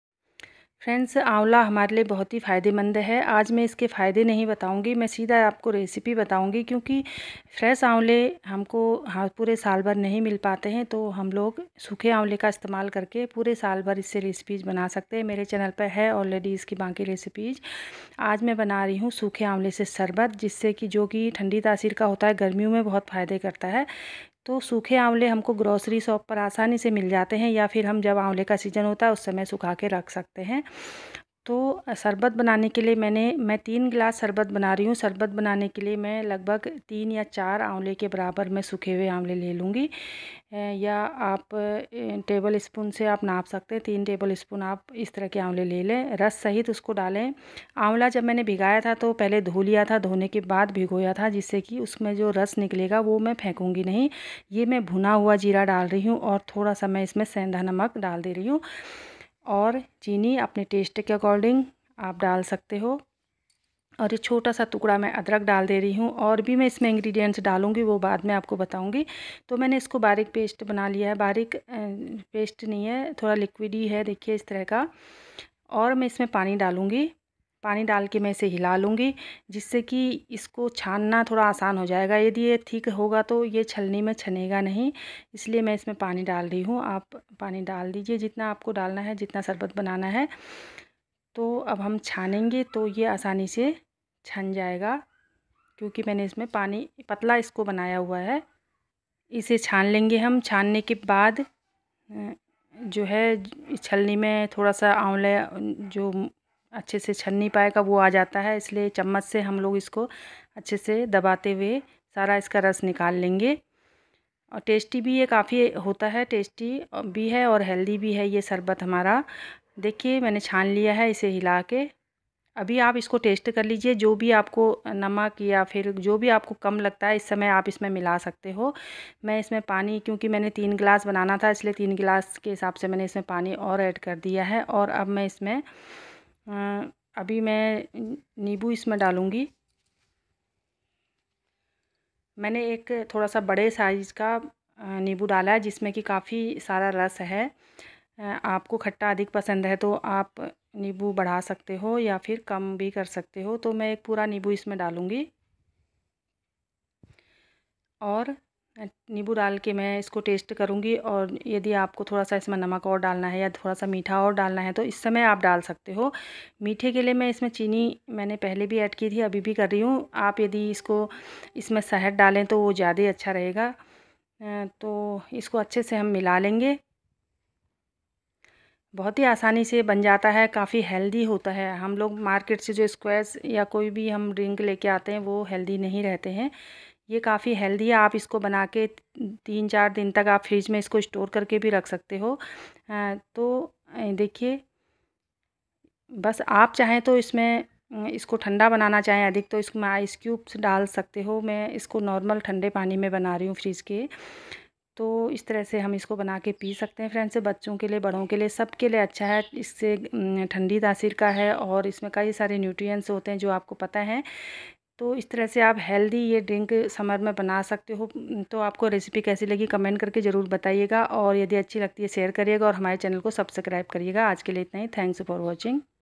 सूखे आंवले का शरबत | dry amla recipe | ठंडी तासीर वाला शरबत | summer drink|garmiyon ka sharbat
दोस्तों! आज मैं सूखे आंवले से शरबत की रेसिपी बना रही हूं। जो आपको बहुत ज्यादा पसंद आने वाली है। क्योंकि यह टेस्टी तो है ही साथ में हेल्दी भी काफी है।
My Channel Live Up Life is All About Daily Blogging/ New DIY Ideas/ Home Decor/
Kitchen & Gardening Tips/ New breakfast recipe,